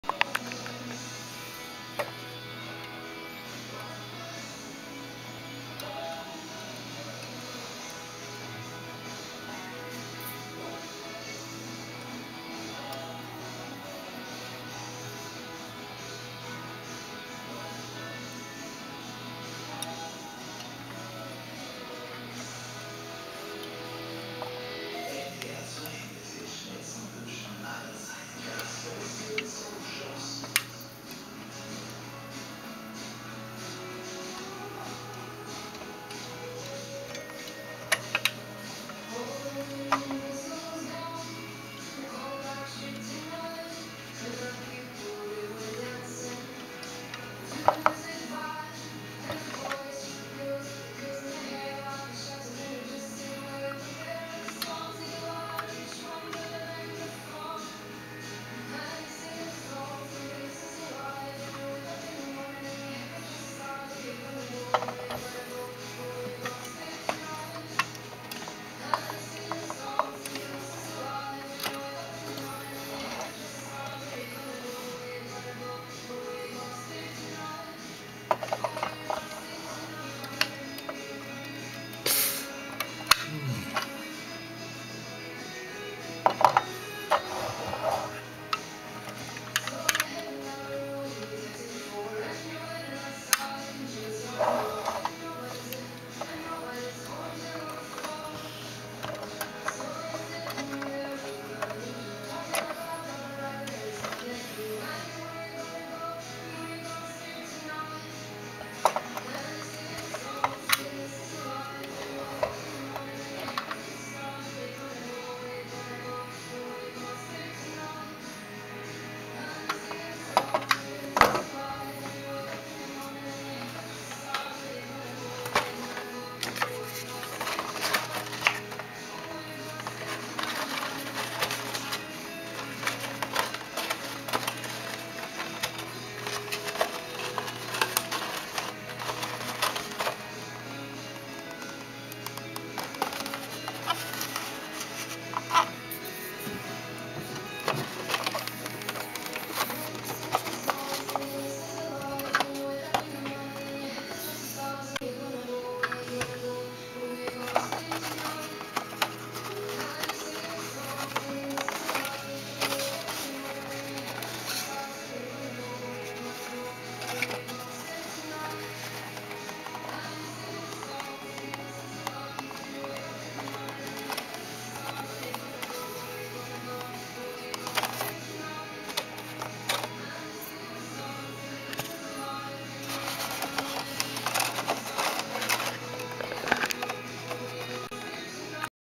Guisval Seat/Fiat 124 Policia scala 1/23 mint boxed
I´m showing here my mega rare 1/23rd scale Seat model from Guisval, which I´m selling on ebay: article number is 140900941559

a video is more worth than hundreds of pictures!
The model has tiny chips which you have to search carefully to find them, a damged box and a little crack in the frontgrille.
I haven´t cleaned it so it might look much better when you polish it but be aware to the paperdecals...